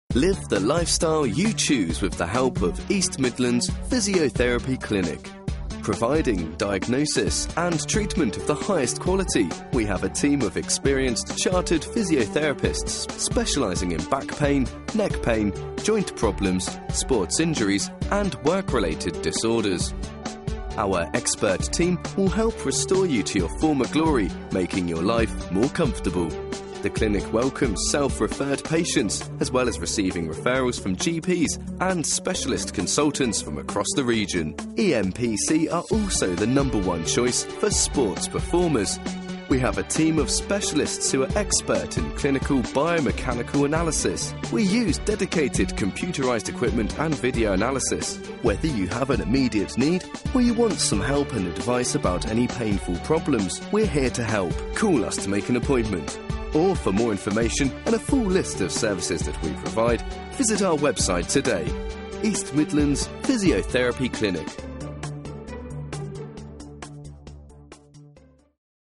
East Midlands Physiotherapy Clinic
We are a private physiotherapy clinic providing diagnosis and treatment of the highest quality.  We have a team of experienced Chartered Physiotherapists specialising in back pain, neck pain, joint problems, sports injuries and work related disorders.

Why Choose EMPC?

-         A choice of Specialist Physiotherapists

-         Outstanding clinic facilities

-         Close links with GP's and Consultants

-         Orthotic and Pilates Services

-         A warm welcome guaranteed

We are the Choice of:

-         Leicestershire County Cricket Club

-         The FA Medical Centre

-         Premiership and League Football Clubs

All our physiotherapists are registered with the Health Professionals Council and Members of the Chartered Society of Physiotherapy.

We are recognised by all major insurance companies.